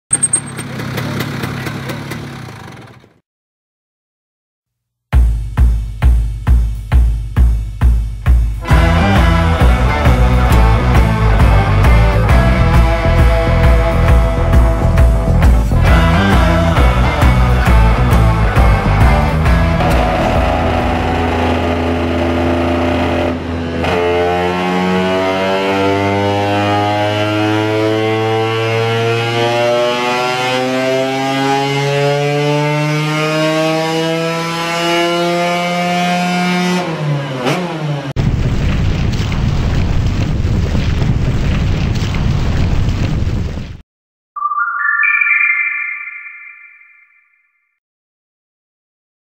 SLUK | Casa Performance Lambretta 250 Elite dyno run
A dyno pull at Rimini Lambretta Centre's Open day of the new Casa Performance '250 Elite' kit designed and by DEA. At the time of running the engine was freshly built in front of customers and using DEA's straight development expansion chamber.